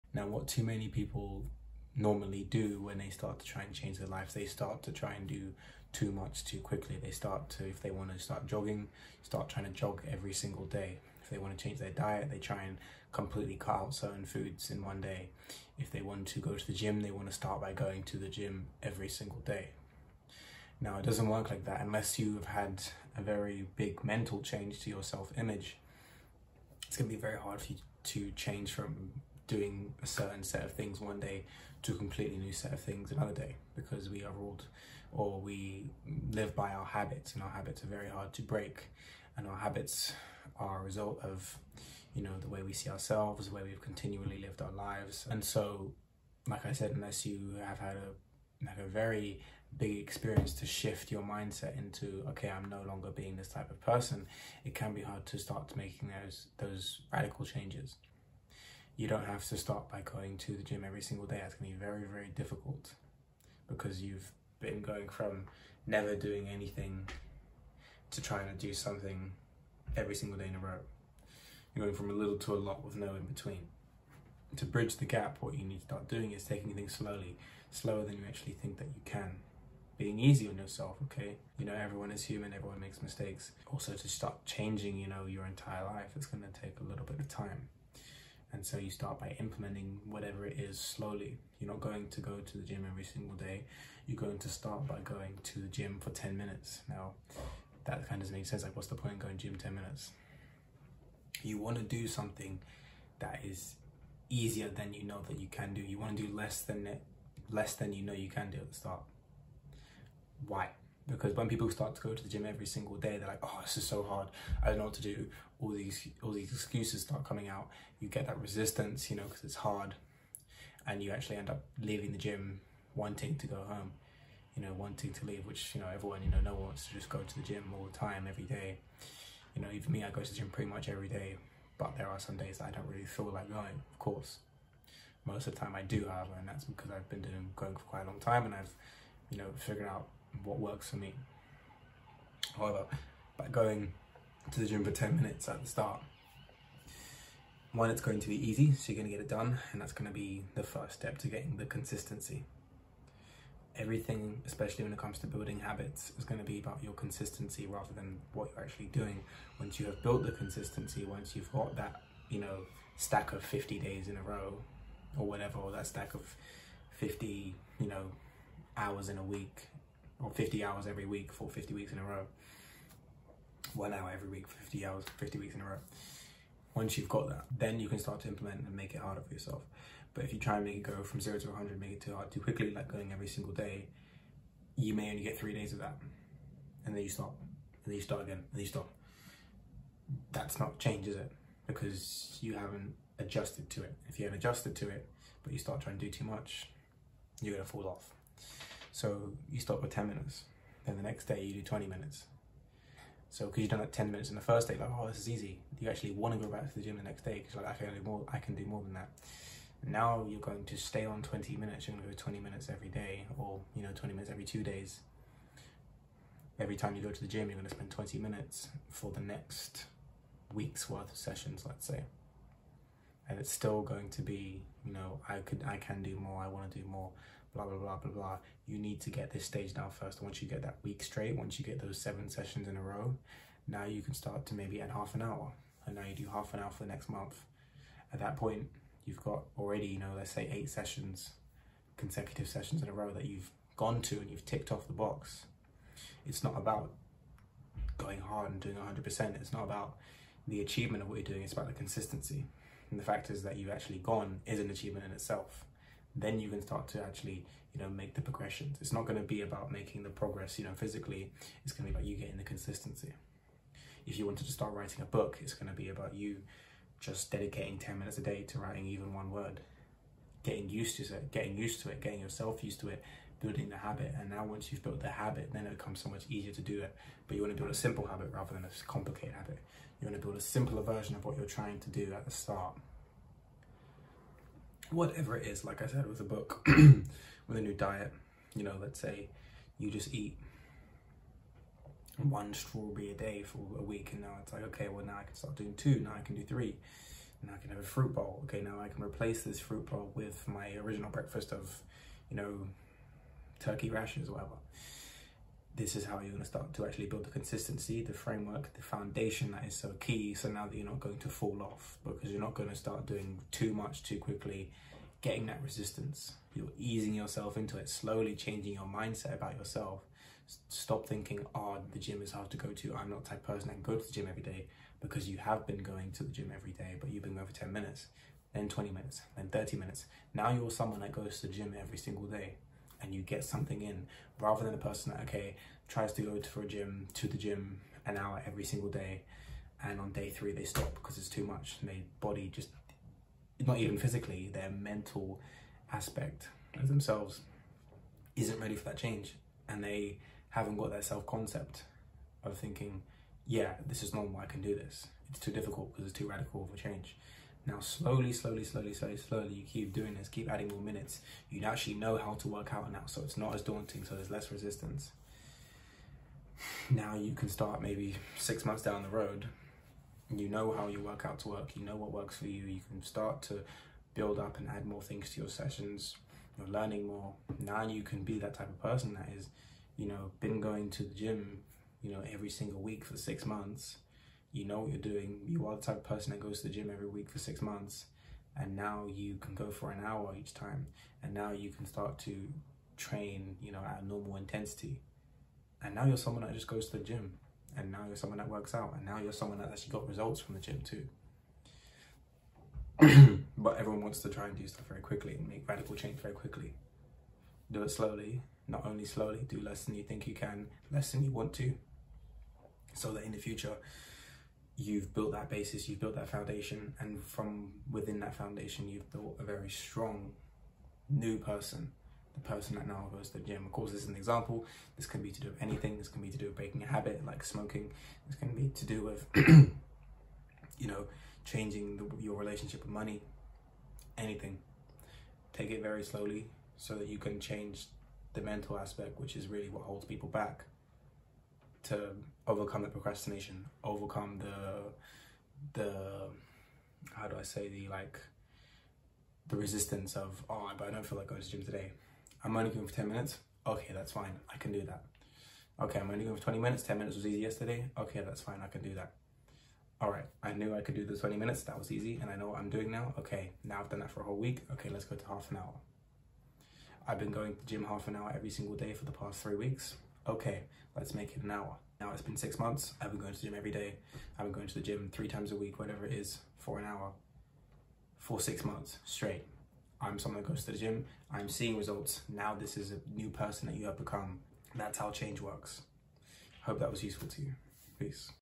How to make changes that last.
Struggling to make big changes to your life? If you want to finally make that breakthrough and build lasting habits this is the video for you...

I am using the gym as an example, but this method can be applied to anything.

Go make some massive changes.